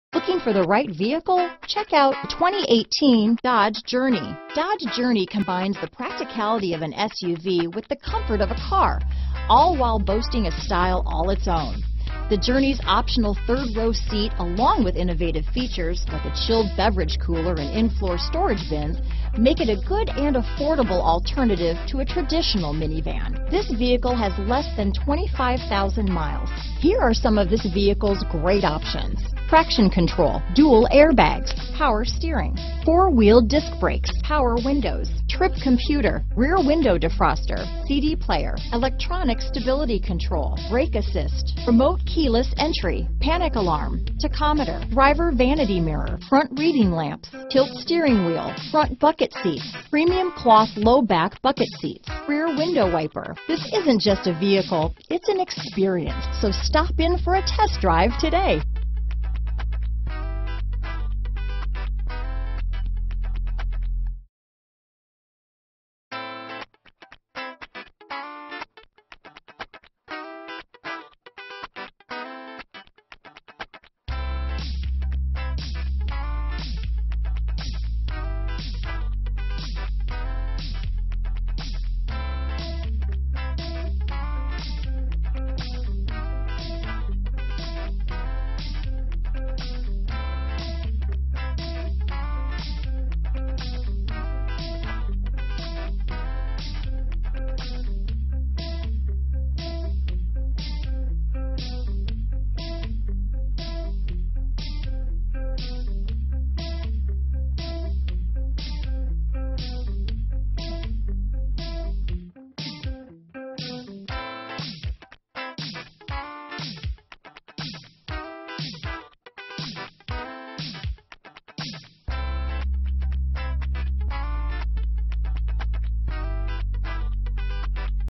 2018 Dodge Journey near me Libertyville, Glenview Schaumburg, Crystal Lake, Arlington Heights, IL 20
Vice White Used 2018 Dodge Journey available near me in Libertyville, Illinois at Napleton Mazda Libertyville. Servicing the Glenview, Schaumburg, Crystal Lake, Arlington Heights, IL area.
2018 Dodge Journey SE - Stock#: 20490A - VIN#: 3C4PDCAB7JT218980